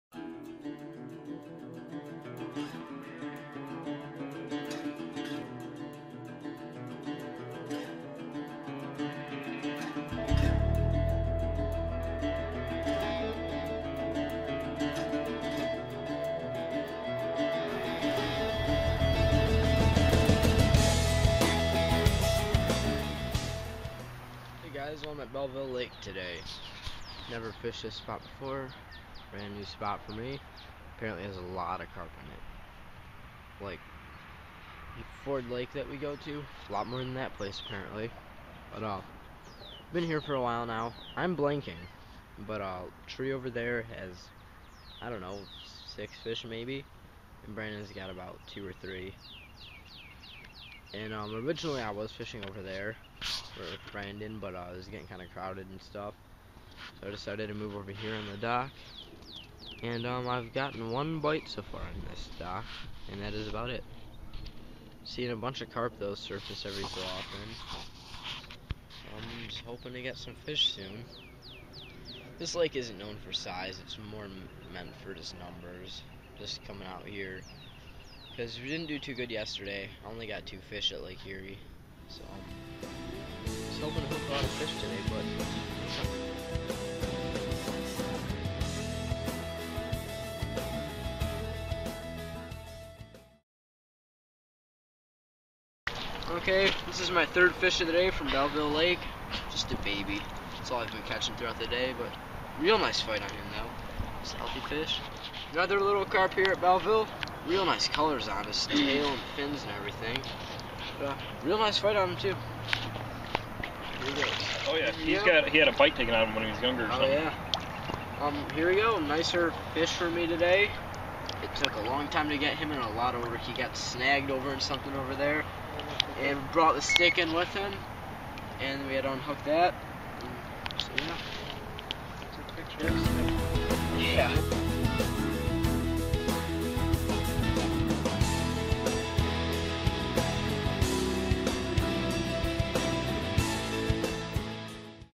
Short Day Fishing at Belleville Lake.
I headed down to bellville lake for a short day after only catching two fish and winning our fish-in due to bad weather and cold fronts shutting the fish off. We came here even though the fish are off this place they are still biting a little bit. I met up with brandon and Andy AKA (Tree) and i got 5 carp and he got 11 and brandon got 6.